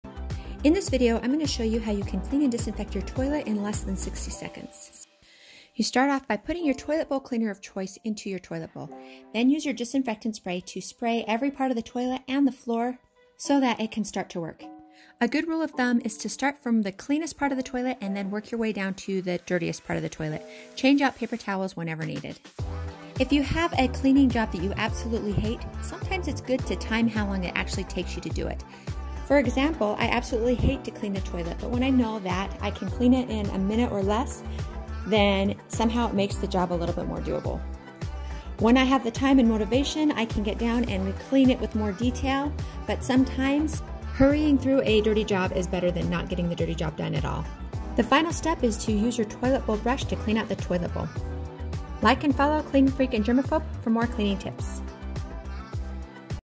Clean a Toilet in 60 Seconds
In this video, you watch a toilet get cleaned and 60 seconds. If you have a daily or weekly job that you absolutely hate to do, time how long it actually takes to do it. Sometimes when you can quantify the task, it makes it much easier to tackle.